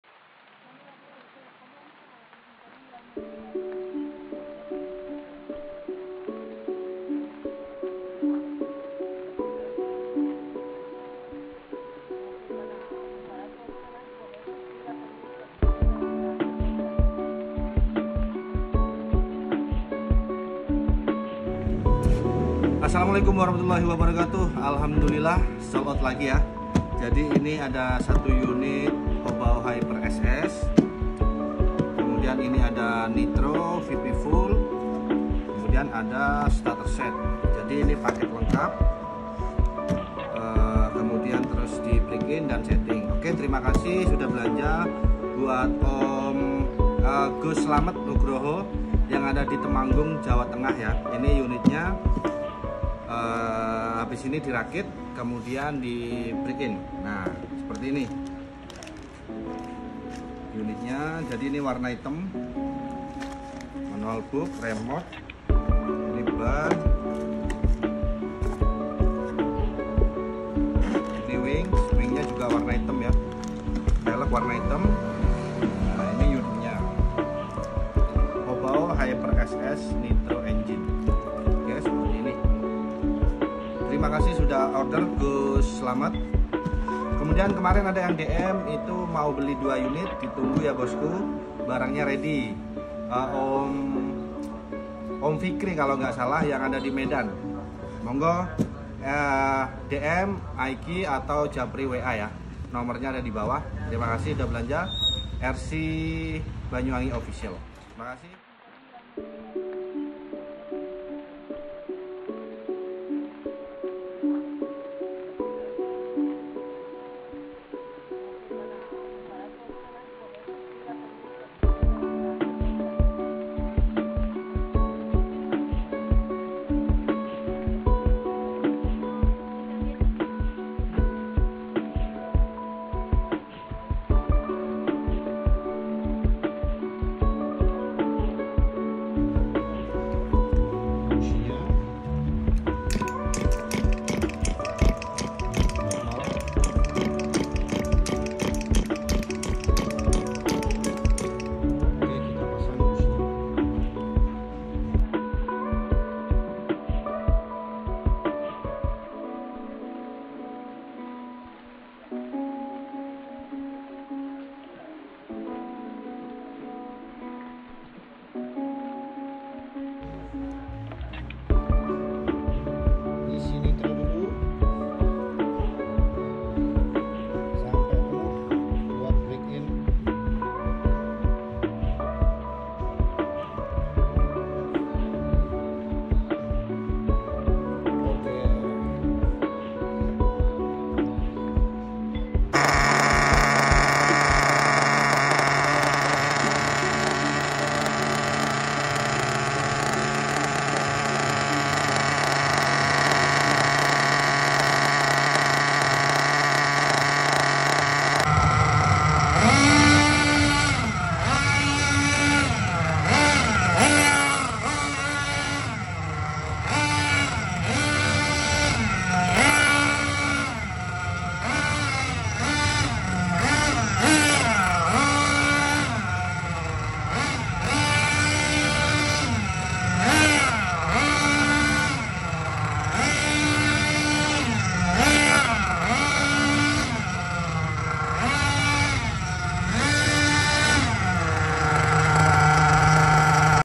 HOBAO HYPER SS NITRO ENGINE SOLD OUT!
Sold out
Terima kasih GUS SLAMET
yang ada di Temanggung JATENG
kita break in dan setting dulu ya GUS...